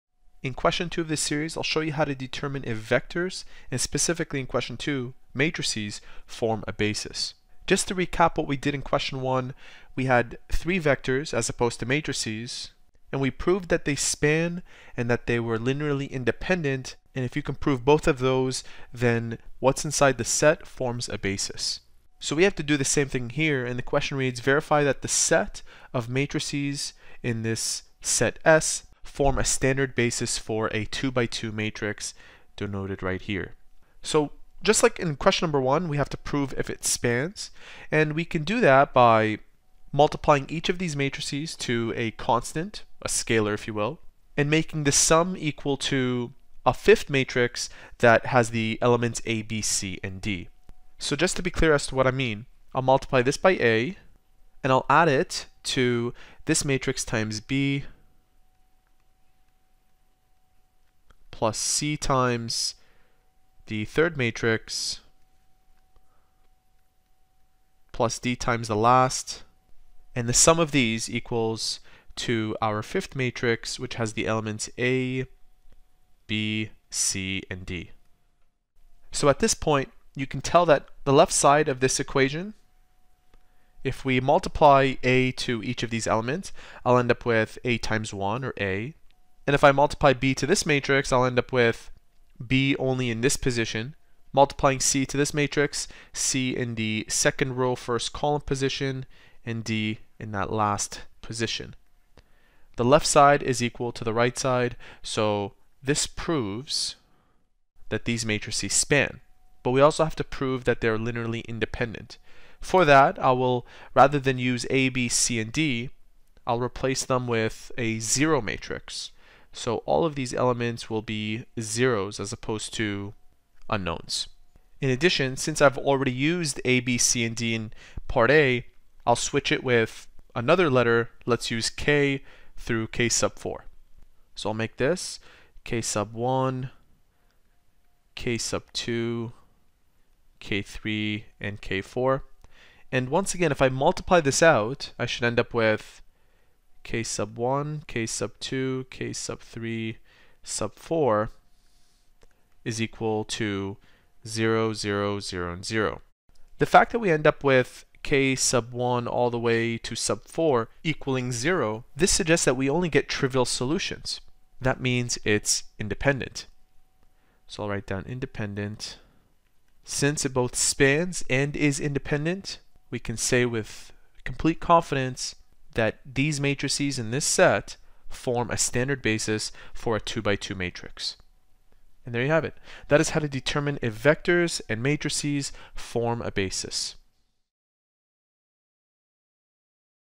📚 Determine if matrices form a basis (Question 2)
Q1.   Determine whether the vectors (1, -3, 2), (-4, -1, 0), and (-1, 2, 1) form a basis for R3.

Q2.   Verify that the set S={m_11,m_12,m_21,m_22 }  comprised of the matrices shown below forms a standard basis for M_22.